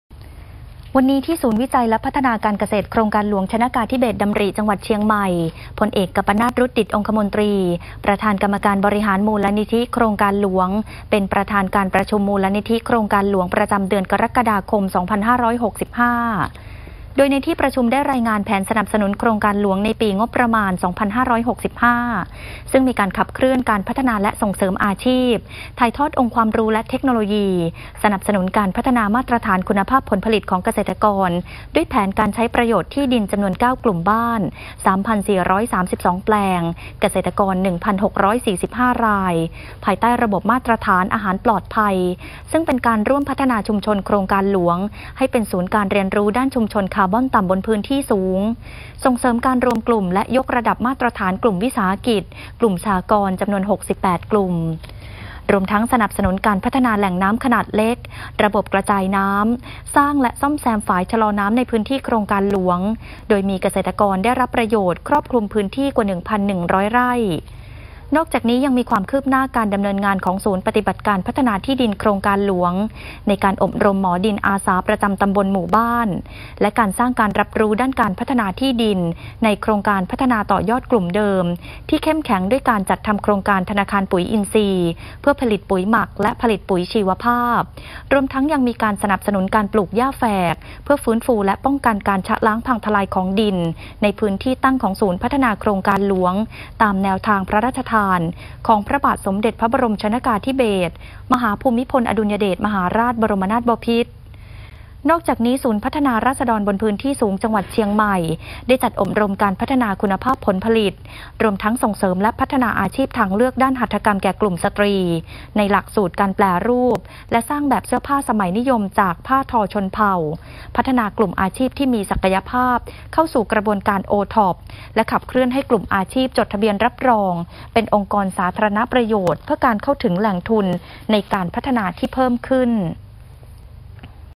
องคมนตรี ประชุมติดตามการดำเนินงานมูลนิธิโครงการหลวง ประจำเดือนกรกฎาคม 2565
ที่ศูนย์วิจัยและพัฒนาการเกษตรโครงการหลวง ชนกาธิเบศรดำริ อำเภอเมือง จังหวัดเชียงใหม่ พลเอก กัมปนาท รุดดิษฐ์ องคมนตรี ประธานกรรมการบริหารมูลนิธิโครงการหลวง เป็นประธานการประชุมมูลนิธิโครงการหลวง ประจำเดือนกรกฎาคม 2565 ผ่านสื่ออิเล็กทรอนิกส์ เพื่อรับฟังความคืบหน้าการดำเนินงานของหน่วยงานที่เกี่ยวข้อง ซึ่งสถาบันวิจัยและพัฒนาพื้นที่สูง ได้รายงานแผนสนับสนุนในปีงบประมาณ 2565 โดยร่วมขับเคลื่อนการวางแผนการใช้ประโยชน์ที่ดิน 9 กลุ่มบ้าน 3,432 แปลง เกษตรกร 1,645 คน พัฒนาและส่งเสริมอาชีพ ถ่ายทอดองค์ความรู้และเทคโนโลยี สนับสนุนการพัฒนามาตรฐานคุณภาพผลผลิตใต้ระบบมาตรฐานอาหารปลอดภัย ร่วมพัฒนาชุมชนให้เป็นศูนย์การเรียนรู้ด้านชุมชนคาร์บอนต่ำบนพื้นที่สูง ส่งเสริมการรวมกลุ่มและยกระดับมาตรฐานกลุ่มวิสาหกิจ กลุ่มสหกรณ์ 68 กลุ่ม สนับสนุนการพัฒนาแหล่งน้ำขนาดเล็ก ระบบกระจายน้ำ สร้างและซ่อมแซมฝายชะลอน้ำ มีเกษตรกรได้รับประโยชน์ 364 ครัวเรือน ส่วนศูนย์ปฏิบัติการพัฒนาที่ดินโครงการหลวง มีการอบรมหมอดินอาสาประจำตำบล-หมู่บ้าน สร้างการรับรู้ด้านการพัฒนาที่ดิน ในโครงการพัฒนาต่อยอดกลุ่มเดิมที่เข้มแข็ง 10 กลุ่ม ในพื้นที่ของศูนย์พัฒนาโครงการหลวง 8 แห่ง จัดทำโครงการธนาคารปุ๋ยอินทรีย์ เพื่อผลิตปุ๋ยหมัก และปุ๋ยชีวภาพ สนับสนุนการปลูกหญ้าแฝกเพื่อฟื้นฟูและป้องกันการชะล้างพังทลายของดิน ตามแนวทางพระราชทานของพระบาทสมเด็จพระมหาภูมิพลอดุลยเดชมหาราช บรมนาถบพิตร โดยมีเป้าหมายการปลูก 3.8 ล้านกล้า

ส่วนศูนย์พัฒนาราษฎรบนพื้นที่สูง จังหวัดเชียงใหม่ ร่วมบูรณาการการพัฒนาและส่งเสริมอาชีพ อบรมการพัฒนาคุณภาพผลผลิต ได้แก่ การปลูกและการปฏิบัติดูแลรักษาเพื่อการผลิตเมล็ดพันธุ์และผลิตเส้นใยกัญชง หรือ เฮมพ์ การเปลี่ยนยอดอะโวคาโด รวมทั้งส่งเสริมและพัฒนาอาชีพทางเลือกด้านหัตถกรรมในหลักสูตรการแปรรูปและสร้างแบบเสื้อผ้าสมัยนิยมจากผ้าทอชนเผ่า พัฒนากลุ่มอาชีพที่มีศักยภาพเข้าสู่กระบวนการ OTOP และขับเคลื่อนให้กลุ่มอาชีพจดทะเบียนรับรองเป็นองค์กรสาธารณประโยชน์ เพื่อการเข้าถึงแหล่งทุนในการพัฒนาที่เพิ่มขึ้น

ข่าวในพระราชสำนักประจำวันที่ 7 กรกฎาคม 2565